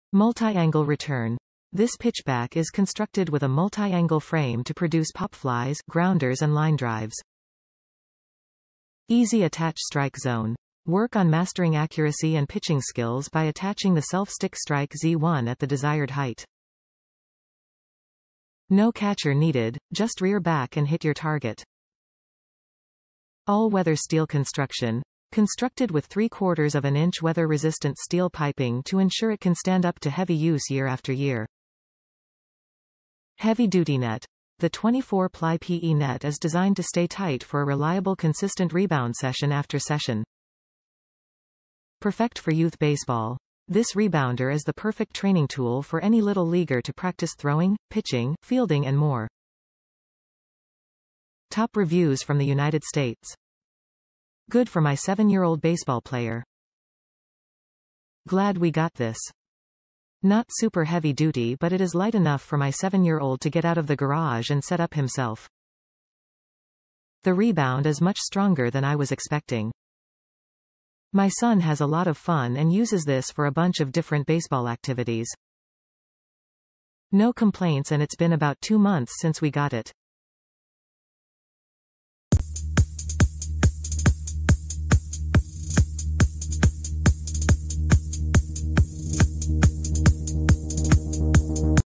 Franklin Sports Baseball Rebounder Net - 3-Way Baseball + Softball Pitchback Net + Fielding Trainer
MULTI ANGLE RETURN: This pitch back is constructed with a multi angle frame to produce pop flies, grounders and line drives
EASY ATTACH STRIKE ZONE: Work on mastering accuracy and pitching skills by attaching the self stick strike z1 at the desired height. No catcher needed, just rear back and hit your target
ALL WEATHER STEEL CONSTRUCTION: Constructed with 3/4 inches weather resistant steel piping to ensure it can stand up to heavy use year after year
HEAVY DUTY NET: The 24 ply PE net is designed to stay tight for a reliable consistent rebound session after session
PERFECT FOR YOUTH BASEBALL: This rebounder is the perfect training tool for any Little Leaguer to practice throwing, pitching, fielding and more

Good for my 7 year old baseball player.
Glad we got this. Not super heavy duty but it is light enough for my 7 year old to get out of the garage and set up himself. The rebound is much stronger than I was expecting. My son has a lot of fun and uses this for a bunch of different baseball activities. No complaints and its been about 2 months since we got it.